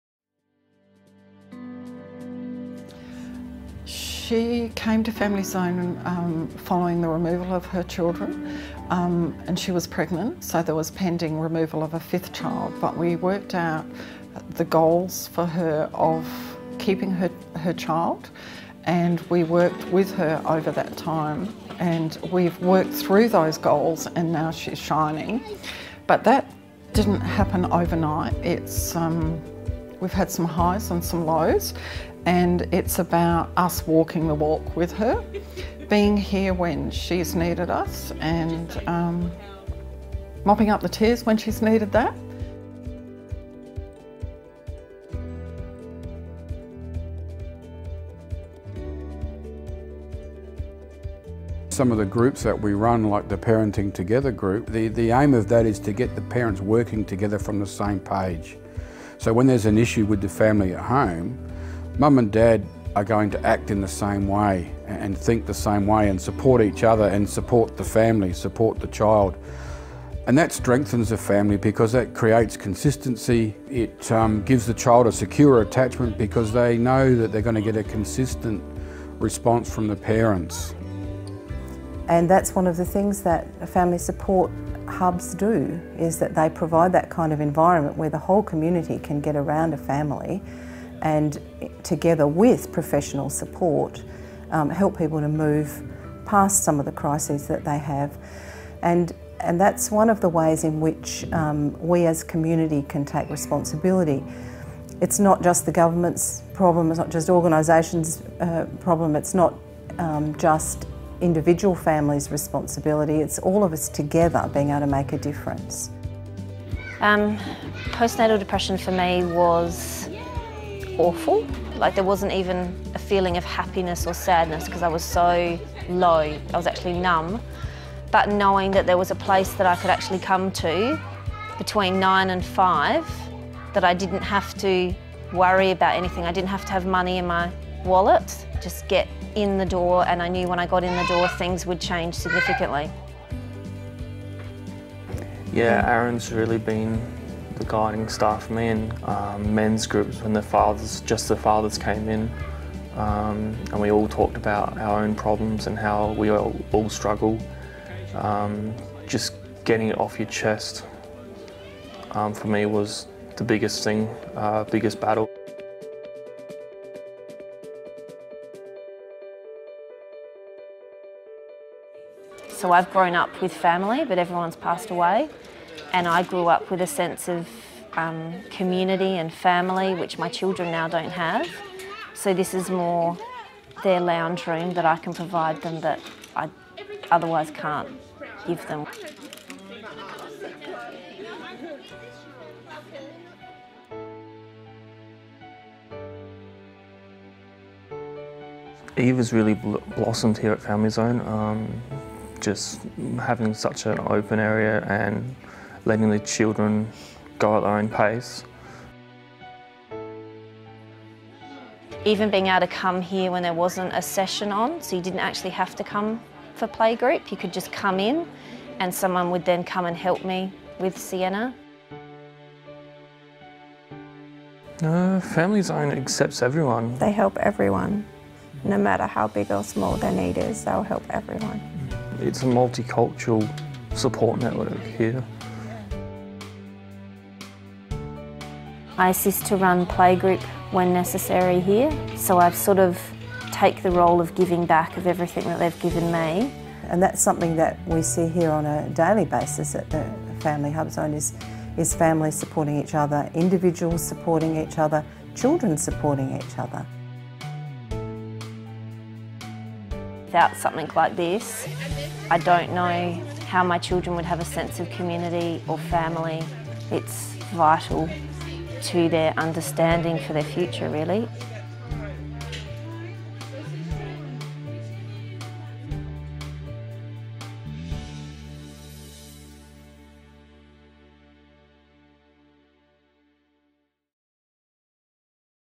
The Family Activity Centre Approach Part 1
A snapshot of a Family Activity Centre in action at Ingle Farm in South Australia